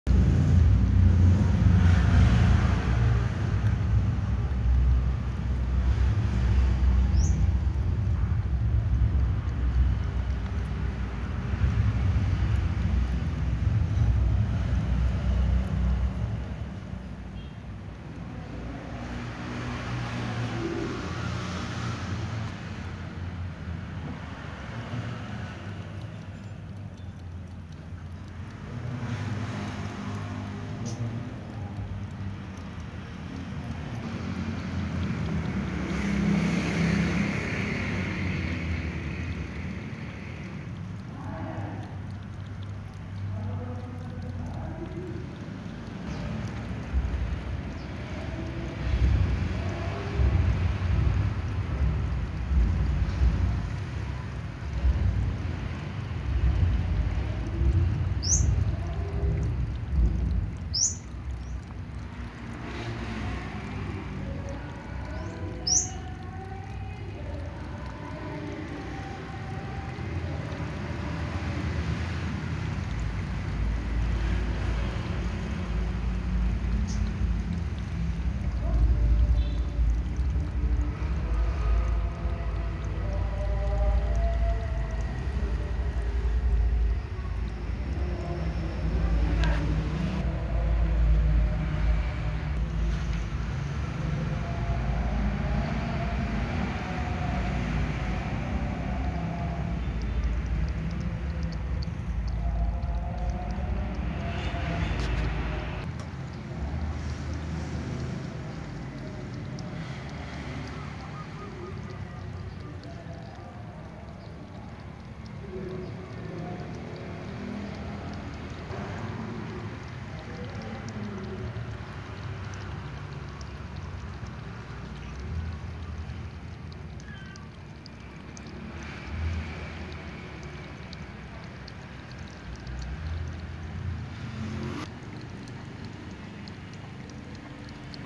the brown cat is drinking water then the white cat is taking over accompanied by another white cat
the brown cat is drinking water then the white cat is taking over accompanied by another white cat  , tom cat and black cat  drinking too